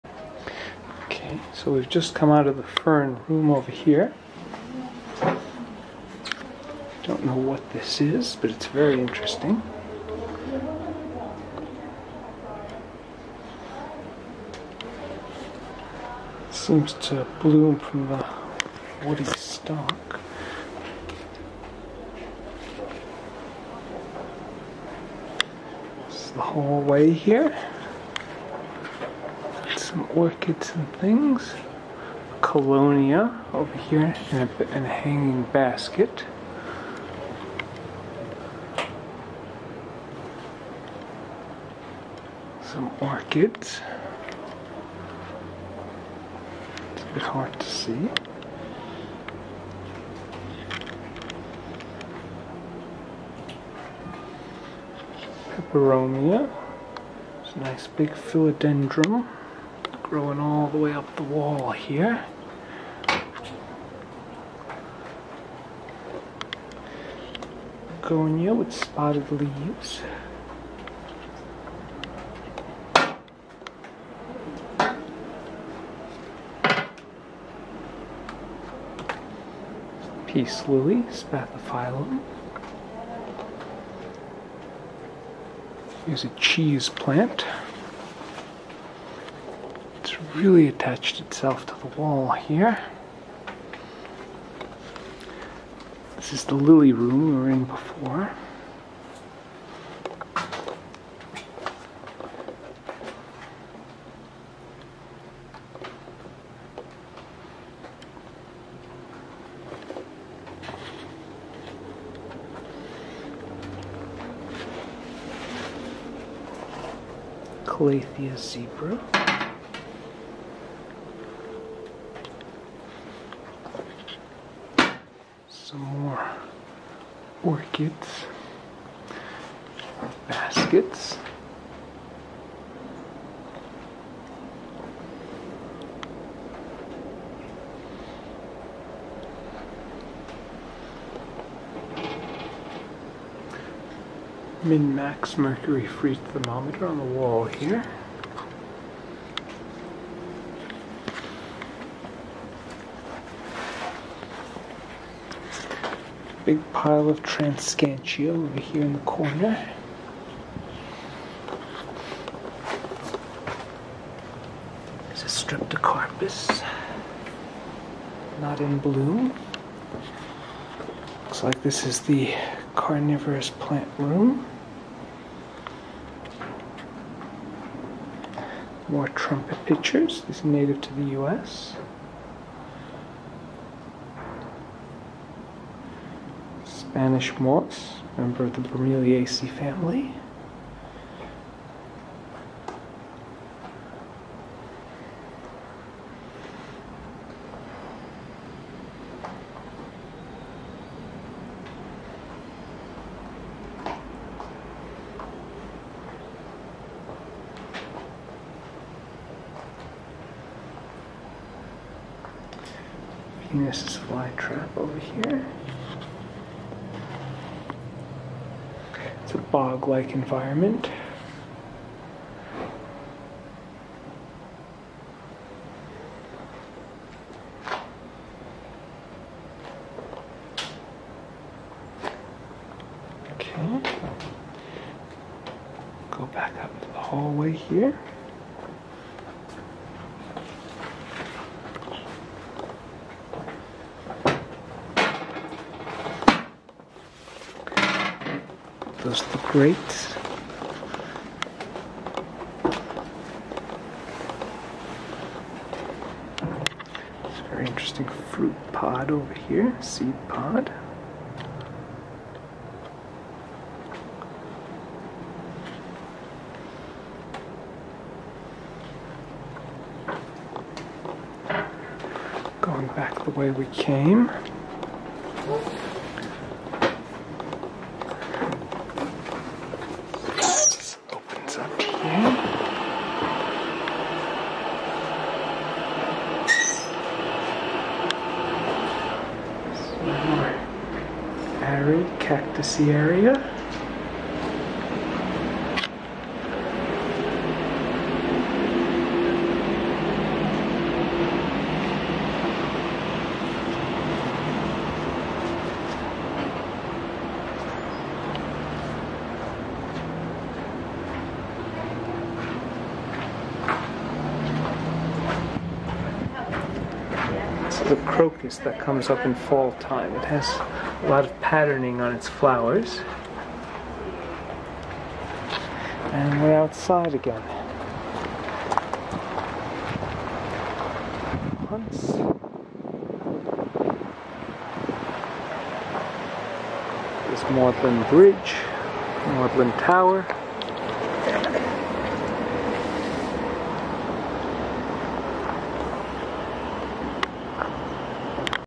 Oxford Botanical Garden 2019 Greenhouse Hallway and Carnivorous Plant Room
Video from my September 2019 trip to the Oxford Botanical Garden showing the greenhouse hallway and carnivorous plant room. The video was filmed with a Canon EOS M3.